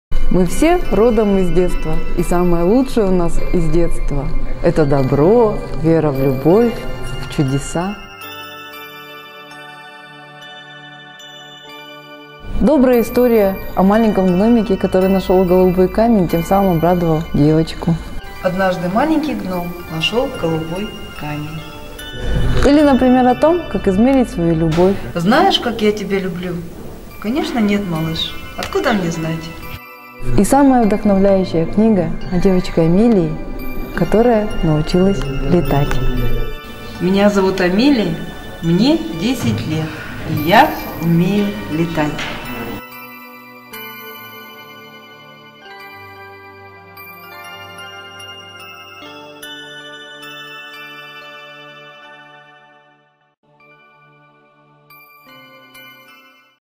Добрые истории на каждый день
Мы все родом из детства…
И самые лучшие качества – доброта, любовь, вера в чудеса, тоже из детства. Испытаем их, читая «Добрые истории на каждый день».
О маленьком гномике, который своей находкой осчастливил девочку из книги «Голубой камень».
О «без размерности» настоящей любви в «Знаешь, как я тебя люблю?»
И вдохновляющая «Амелия, которая умела летать», прочитав которую, вы поймете, что настоящие Мечты сбываются.
Читайте хорошие книги)
Приглашает читальный зал детского сектора библиотеки им.А.С.Пушкина